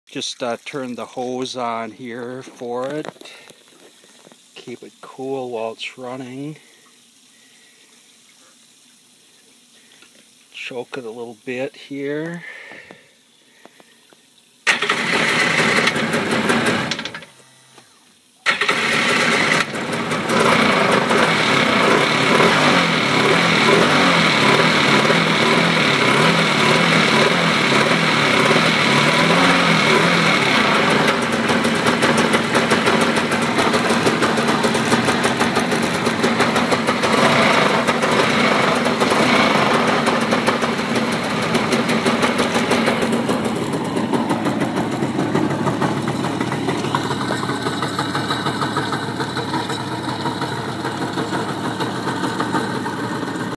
1996 TigerShark Montego
5/23/14 Just prepped with new fuel, hooked up hose to water line and started for the first time this season. compression 180/185 just rebuilt the engine (new rear piston, rings, complete gasket set - also rebuilt carb about a yea ago). only around 10 hours of running last season. runs great.